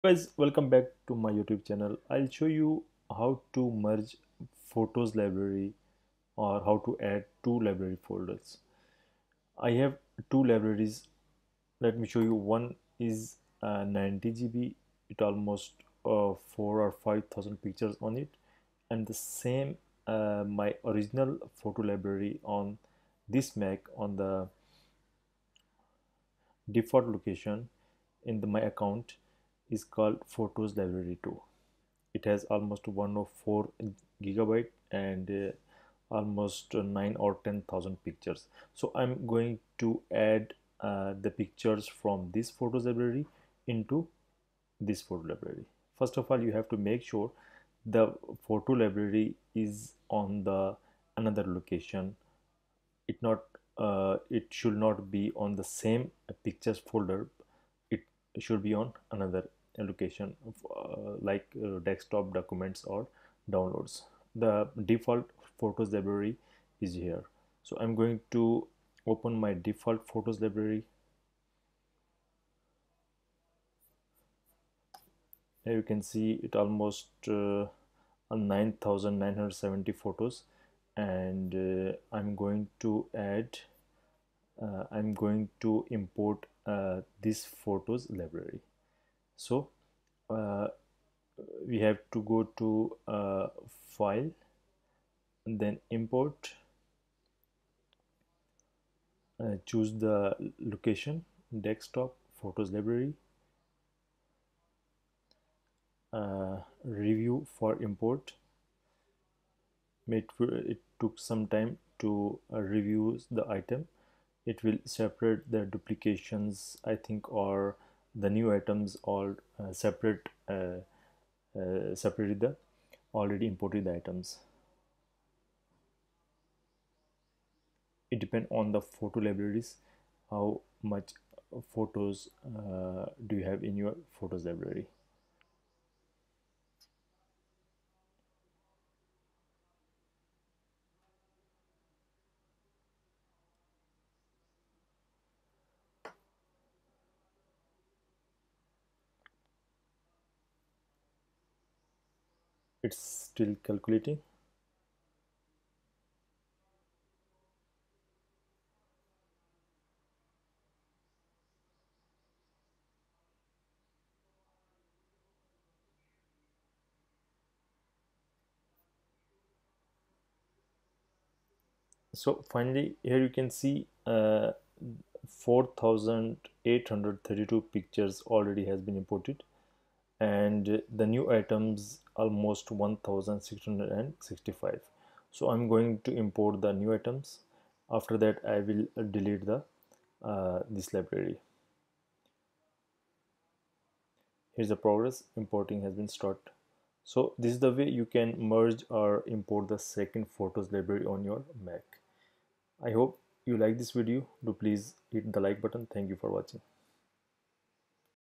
How to import 2nd Photos library on Mac | Merge 2 photos library | Delete Duplicate Photos
If you have tons of photos in two different photos library on Mac and want to merge and find duplicates, watch this video till the end after that you will get free space on your mac.